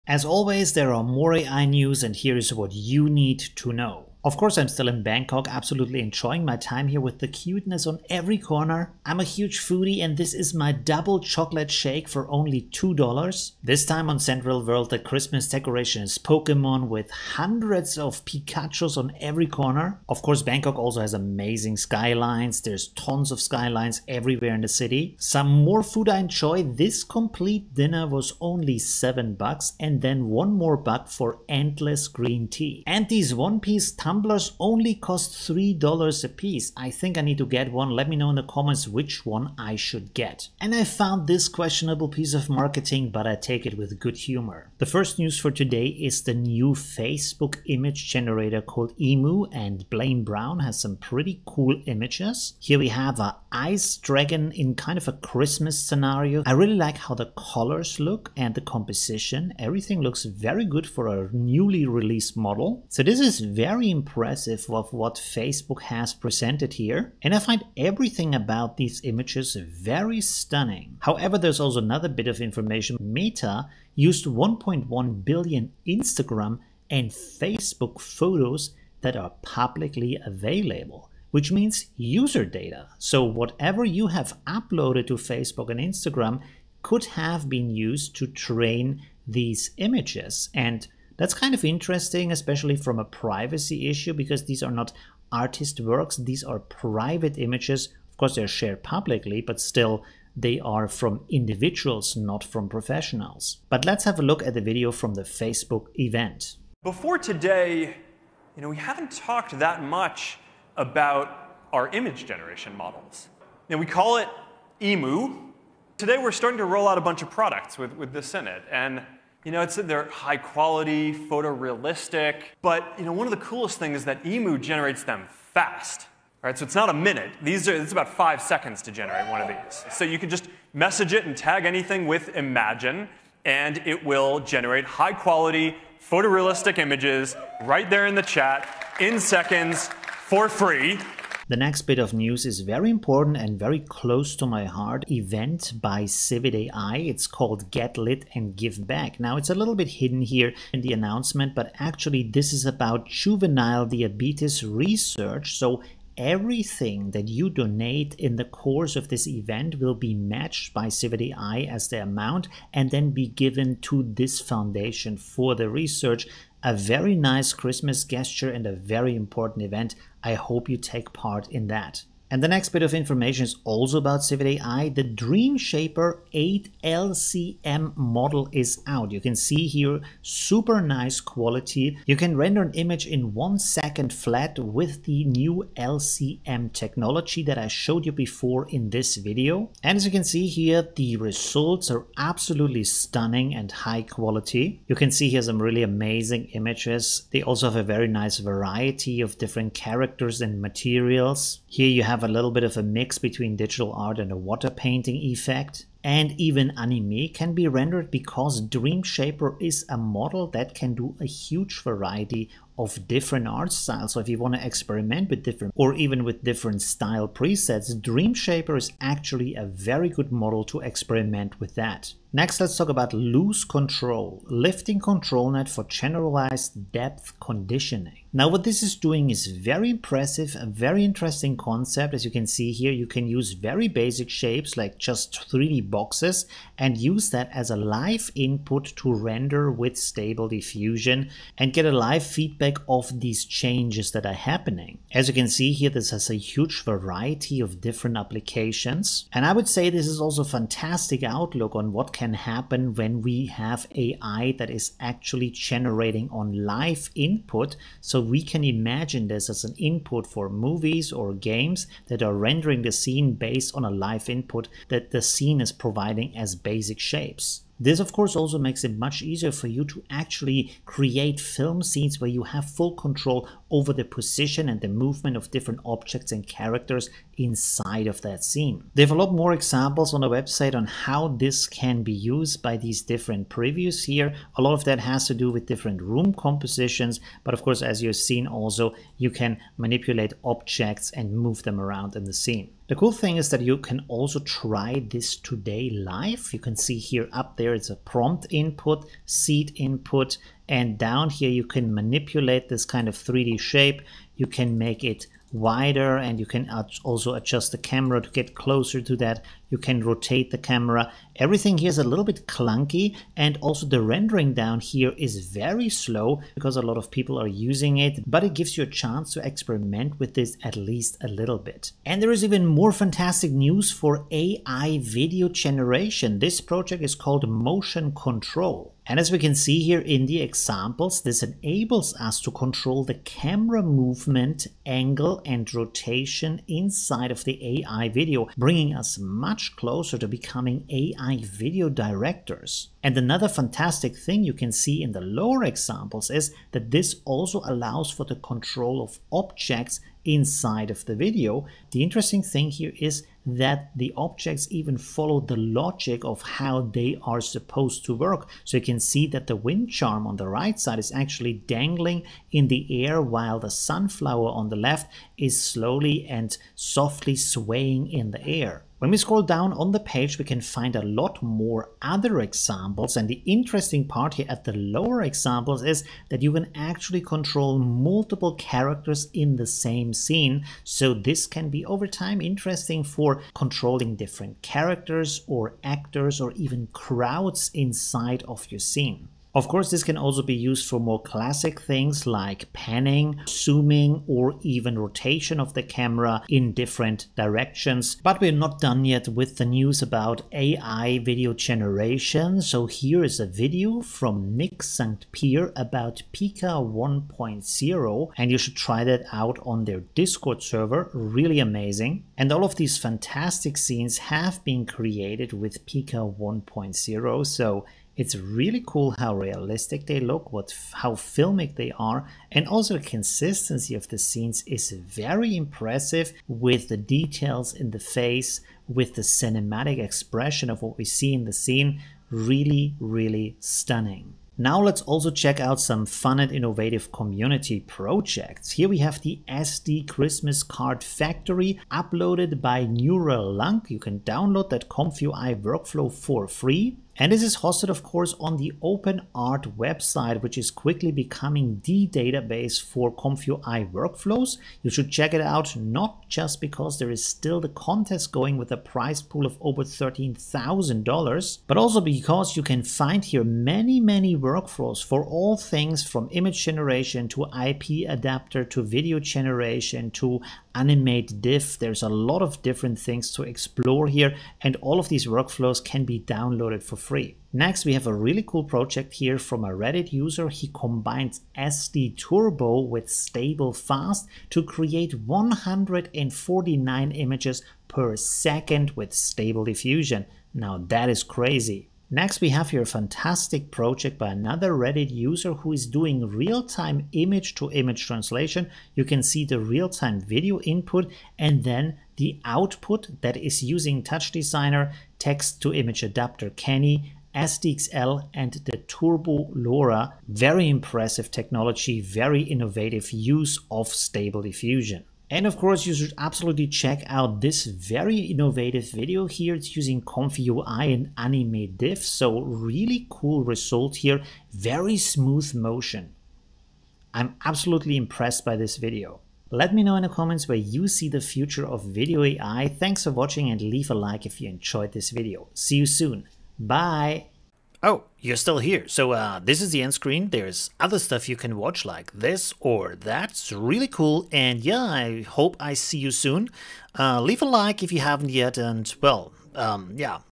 THIS Changes AI Video / Metea EMU uses your Data? AI NEWS
Meta EMU, Pika 1.0, Dreamshaper 8 LCM, CivitAI Diabetes Fundraiser and so much more news!